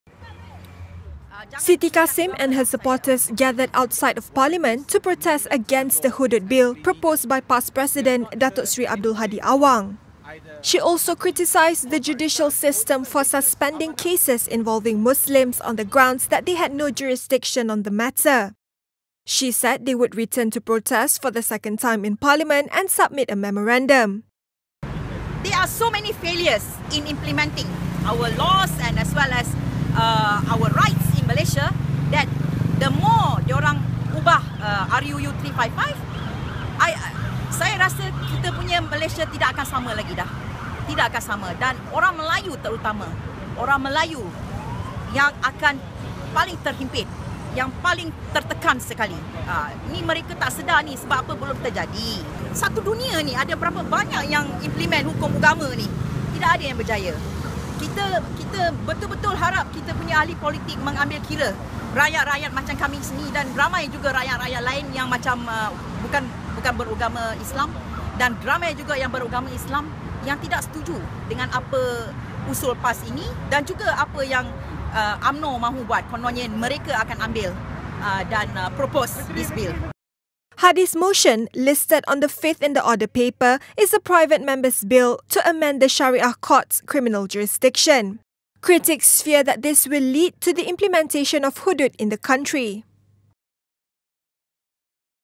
Activist Siti Kasim protests hudud bill outside Parliament
Siti Kasim and her supporters gathered outside of Parliament on Monday to protest against PAS' Private Member's Bill on amendments to the Syariah Courts Act that the party is set to table in the Dewan Rakyat.